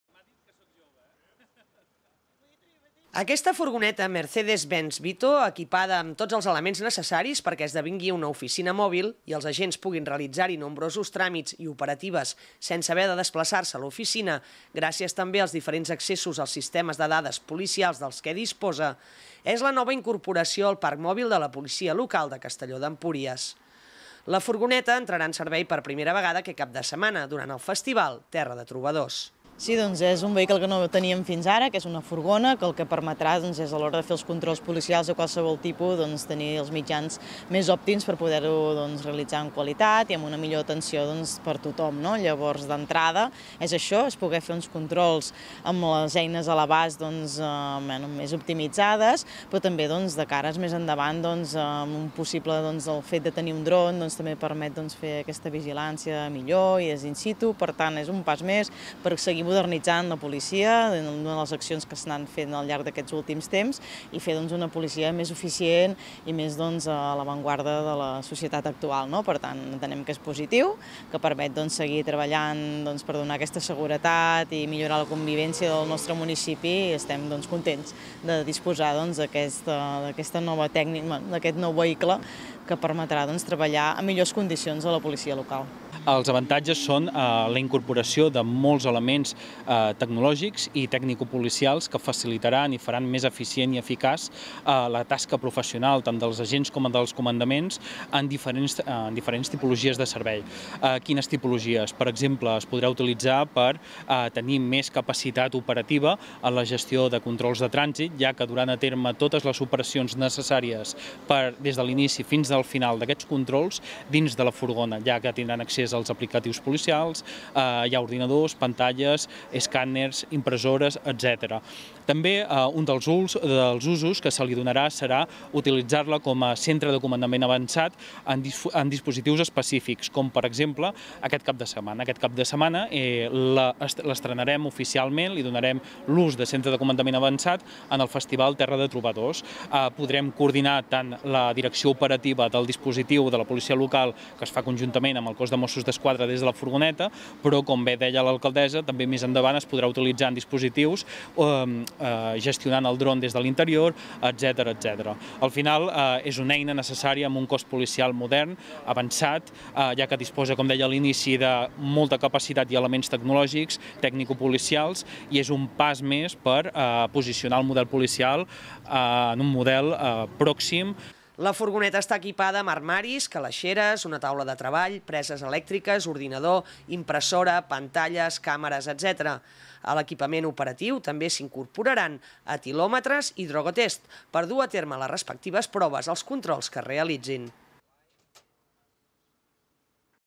La policia local de Castelló presenta la nova furgoneta d’atestats
Aquesta furgoneta Mercedes Benz Vito, equipada amb tots els elements necessaris perquè esdevingui una oficina mòbil i els agents puguin realitzar-hi nombrosos tràmits i operatives sense haver de desplaçar-se a l'oficina, gràcies també als diferents accessos als sistemes de dades policials dels que disposa és la nova incorporació al parc mòbil de la policia local de Castelló d’Empúries. La furgoneta entrarà en servei per primera vegada aquest cap de setmana durant el Festival Terra de Trobadors.
La furgoneta està equipada amb armaris, calaixeres, una taula de treball, preses elèctriques, ordinador, impressora, pantalles, càmeres, etc. A l'equipament operatiu també s'hi incorporaran etilòmetres i drogotest per dur a terme les respectives proves als controls que es realitzin.